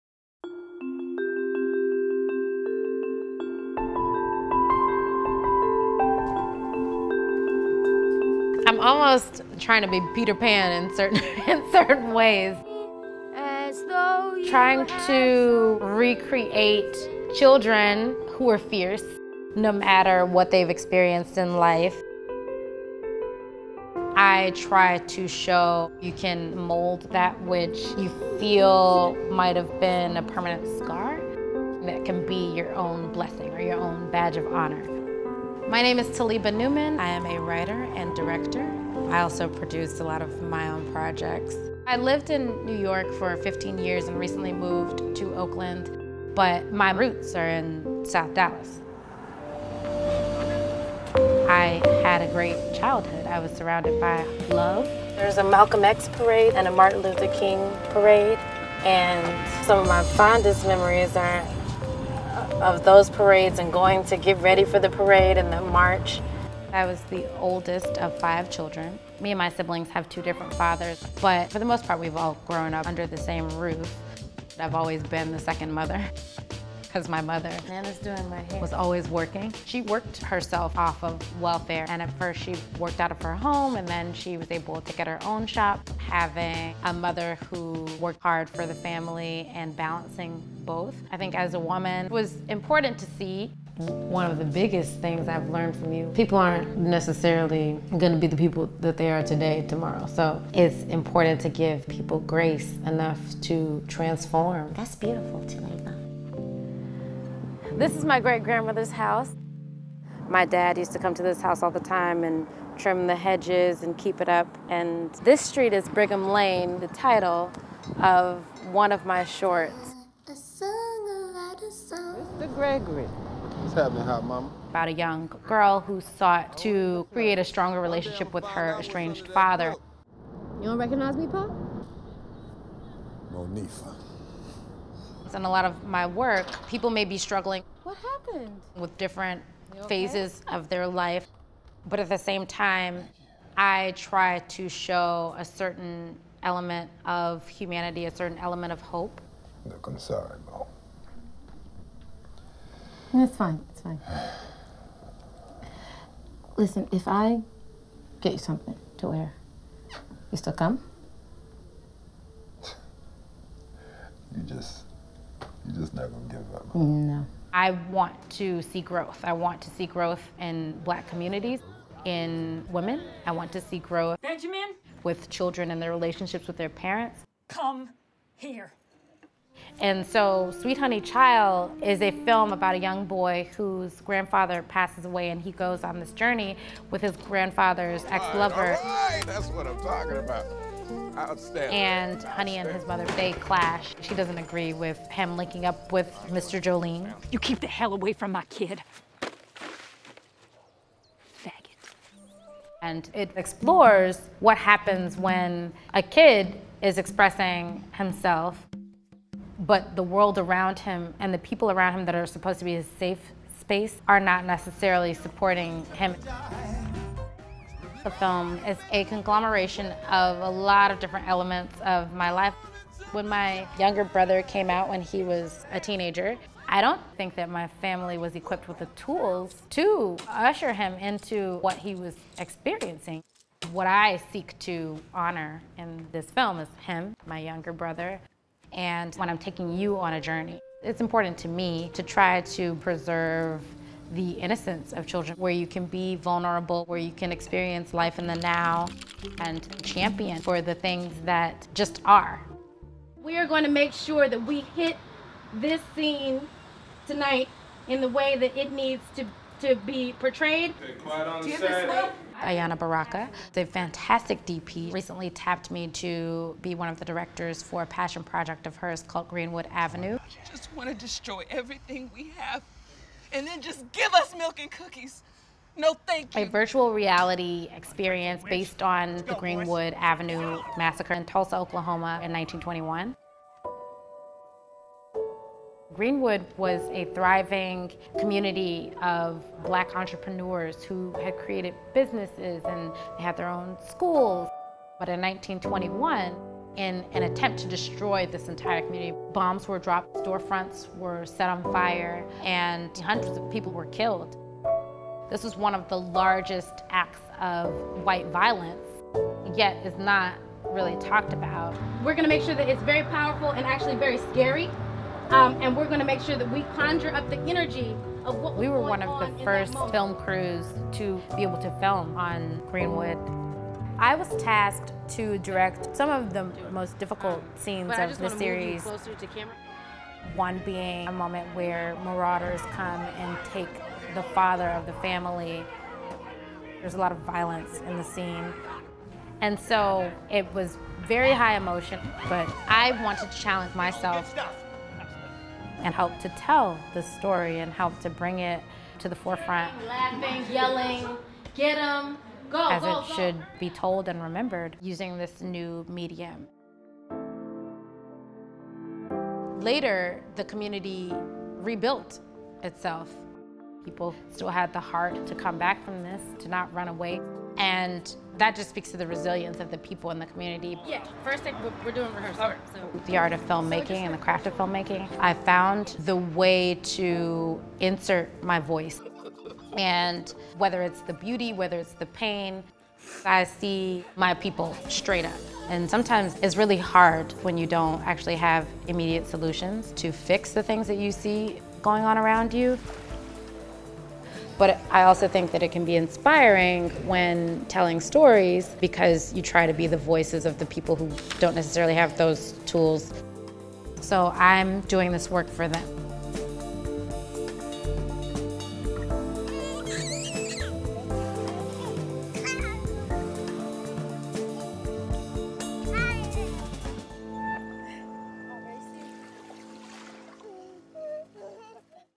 Oakland Filmmaker Tells Stories of Children's Innocence and Resilience | KQED Arts
Black filmmaker and director Talibah Newman doesn’t back down from a challenge. In her 14-year career as a writer, director and producer, she’s worked predominantly with children (no easy task), to tell intimate stories about their innocence and resilience.

“I’m trying to be Peter Pan, in a way,” says the Oakland-based filmmaker, “trying to recreate children who are fierce no matter what they've experienced in life.” Instead of Neverland, those efforts have landed her films at American Black Film Festival and Los Angeles Film Festival, and with distribution by HBO and KQED's Film School Shorts.

Newman believes filmmaking provides her with a space to share a sense of hope—in the possibility of growth, understanding and healing—for the Black community, for women and within families.